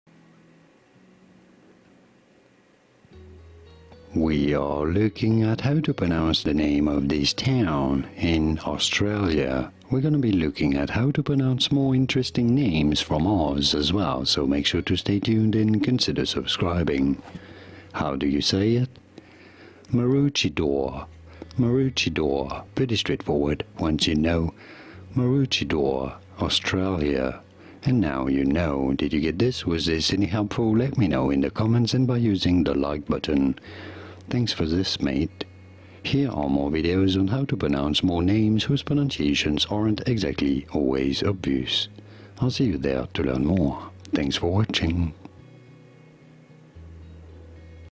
How to Pronounce Maroochydore, Australia? (CORRECTLY)
This video shows you how to pronounce Maroochydore (Australian twon, pronunciation guide).

Information & Source
Maroochydore is a coastal town and suburb of the Sunshine Coast Region, Queensland, Australia. At the 2016 census the suburb recorded a population of 16,800. The town was subdivided from the Cotton Tree reserve by Surveyor Thomas O'Connor in 1903. Wikipedia Source